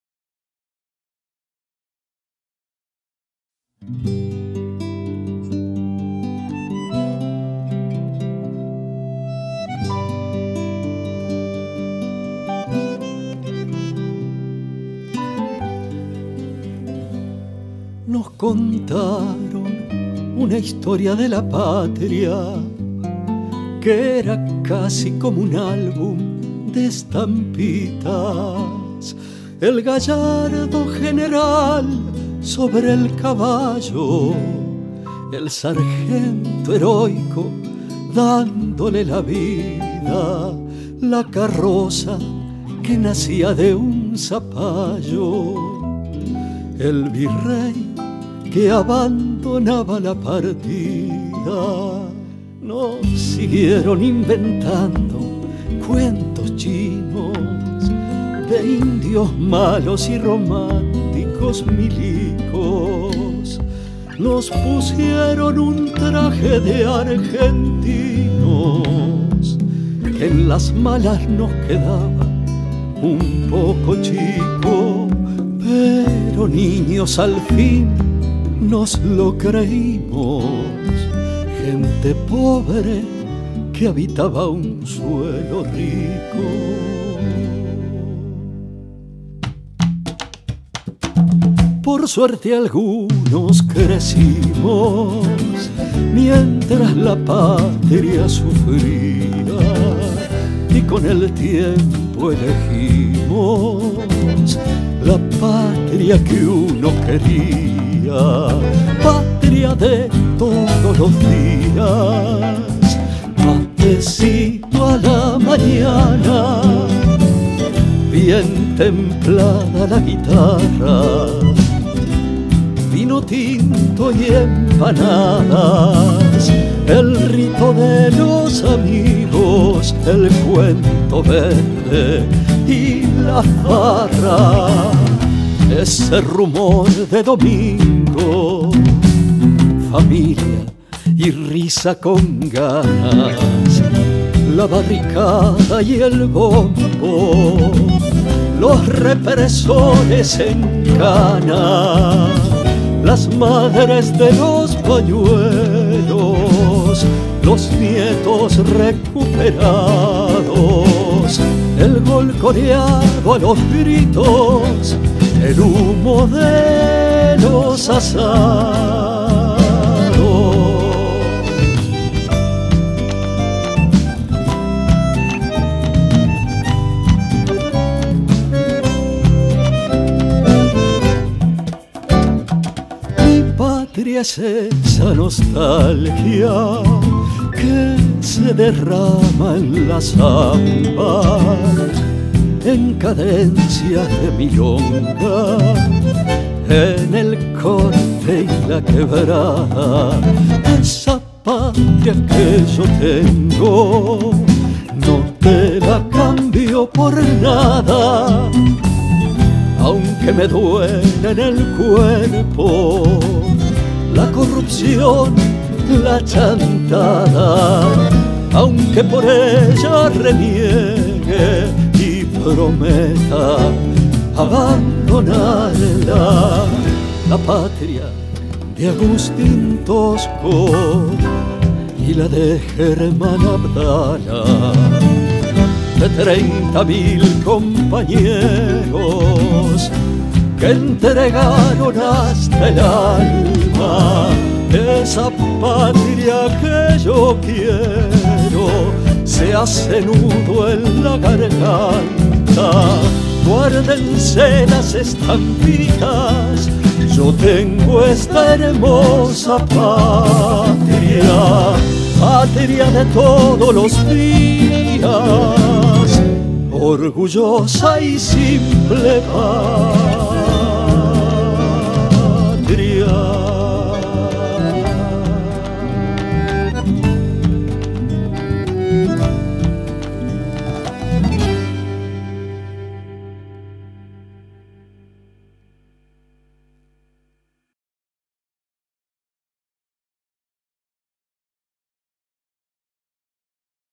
LA PATRIA JUAN MUÑIZ
Nos contaron una historia de la patria
que era casi como un álbum de estampitas:
el gallardo general sobre el caballo,
el sargento heroico dándole la vida,
la carroza que nacía de un zapallo,
el virrey que abandonaba la partida.

Nos siguieron inventando cuentos chinos,
de indios malos y románticos milicos,
nos pusieron un traje de argentinos
que en las malas nos quedaba un poco chico,
pero niños al fin, nos lo creímos:
gente pobre que habitaba un suelo rico.

Por suerte algunos crecimos
mientras la patria sufría
y con el tiempo elegimos
la patria que uno quería.
Patria de todos los días:
matecito a la mañana,
bien templada la guitarra,
vino tinto y empanadas,
el rito de los amigos,
el cuento verde y la farra.

Ese rumor de domingo
familia y risa con ganas,
la barricada y el bombo,
los represores en cana,
las Madres de los pañuelos,
los nietos recuperados,
el gol coreado a los gritos,
el humo de los asados.

Mi Patria es esa nostalgia
que se derrama en la zamba,
en cadencias de milonga,
en el corte y la quebrada.
Esta patria que yo tengo
no te la cambio por nada,
aunque me duela en el cuerpo
la corrupción, la chantada,
aunque por ella reniegue
y prometa abandonarla.

La patria de Agustín Tosco
y la de Germán Abdala,
de 30.000 compañeros
que entregaron hasta el alma,
esa Patria que yo quiero
se hace nudo en la garganta.
Guárdense las estampitas,
yo tengo esta hermosa Patria.

orgullosa y simple Patria.